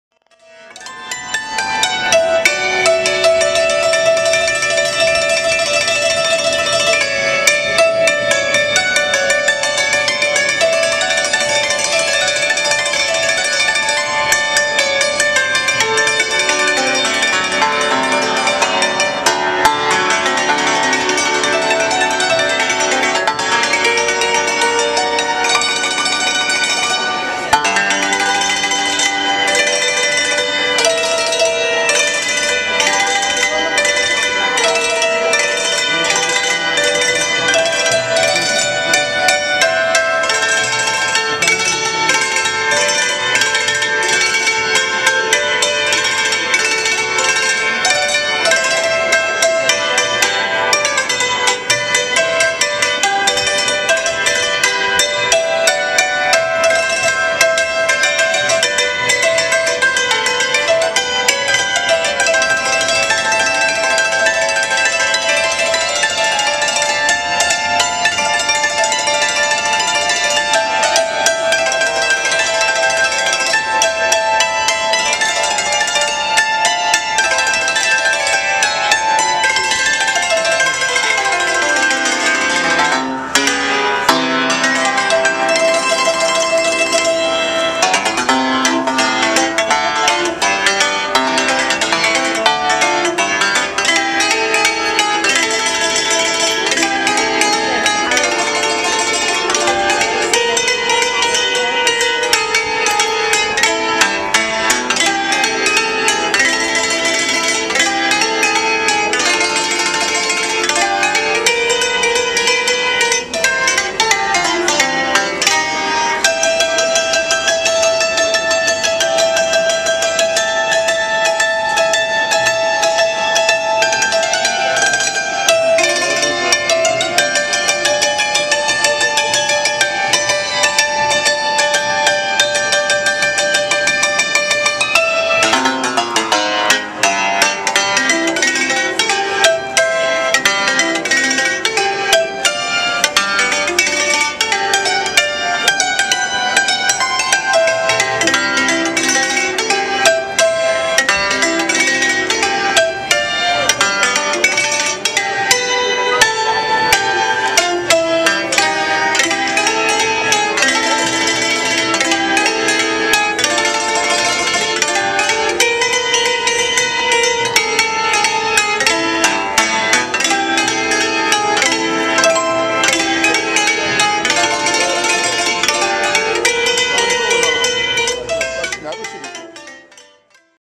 Cimbalom Music played by Miroslav Vavák in Grafton St. Dublin, Ireland
The cimbalom is a concert hammered dulcimer: a type of chordophone composed of a large, trapezoidal box with metal strings stretched across its top. It is a musical instrument popularized in Hungary and commonly found throughout the group of East European nations and cultures which composed Austria-Hungary (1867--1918), namely contemporary Greece, Belarus, Hungary, Romania, Moldova, Ukraine, Poland, the Czech Republic and Slovakia.